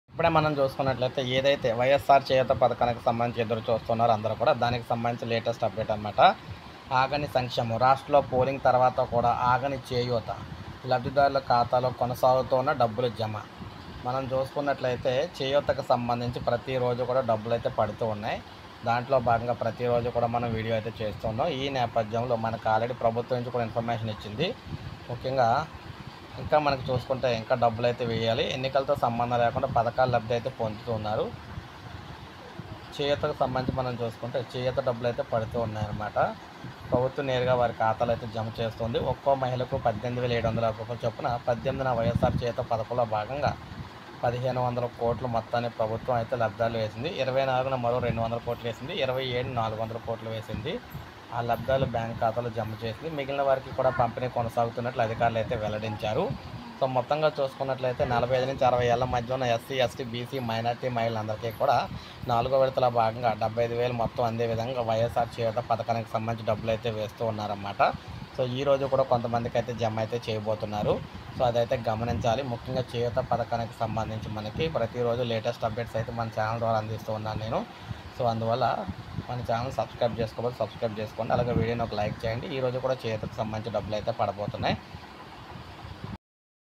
ఇప్పటివరకు ఎంతమందికి చేయూత డబ్బులు వేశారు ప్రకటన YSR cheyutha scheme amount released.
hello everyone today update is AP government YSR cheyutha scheme amount released.@anandsocialedu